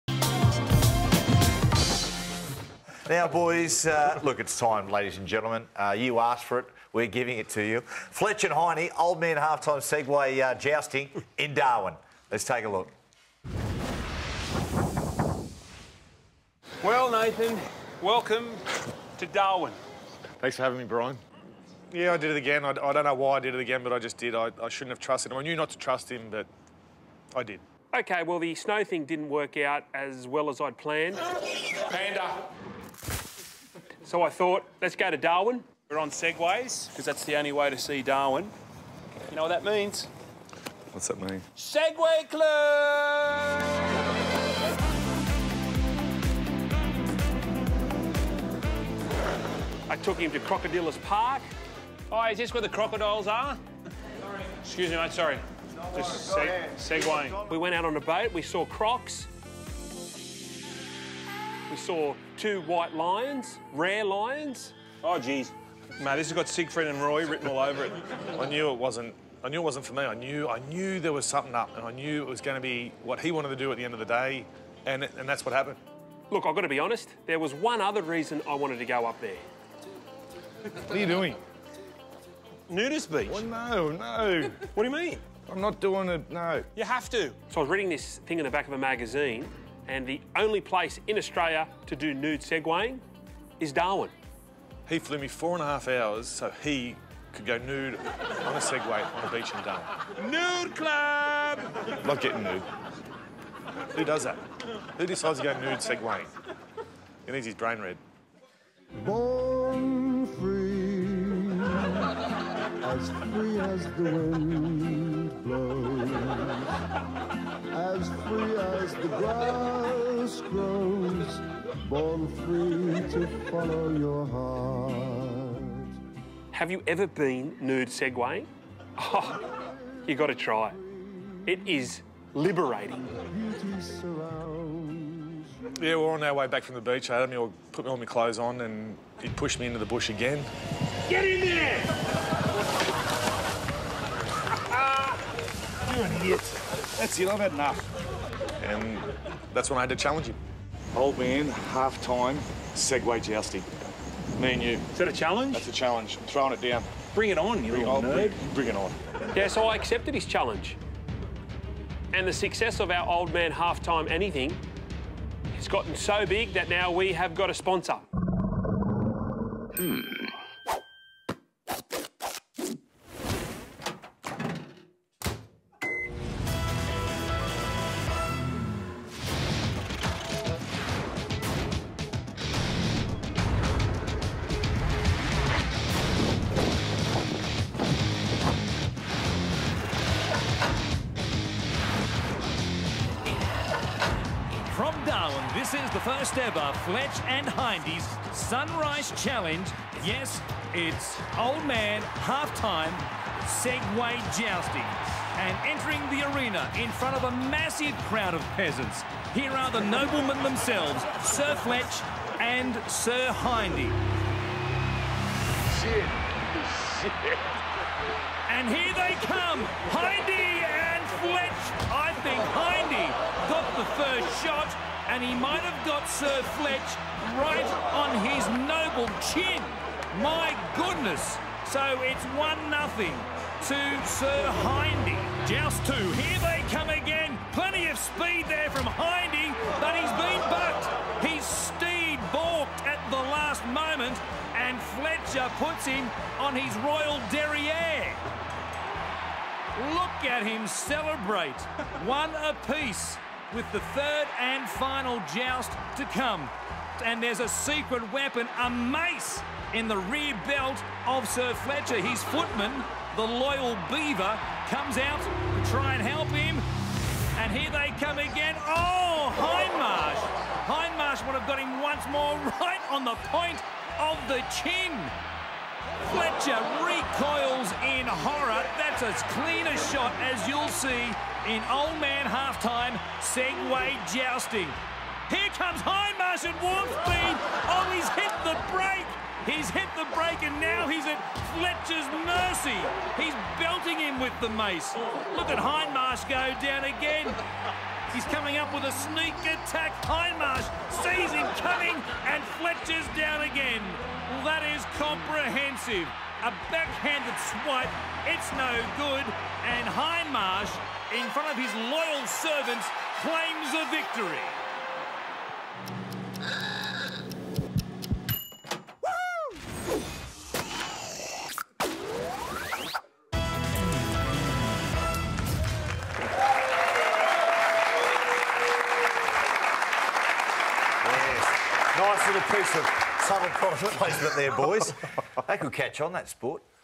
Fletch & Hindy: Segway Jousting
When a trip to Darwin takes a turn for the worse Fletch and Hindy battle it out in some good ol' fashion "Old Man Halftime Segway Jousting"
Watch Sir Fletch and Sir Hindy go toe to toe.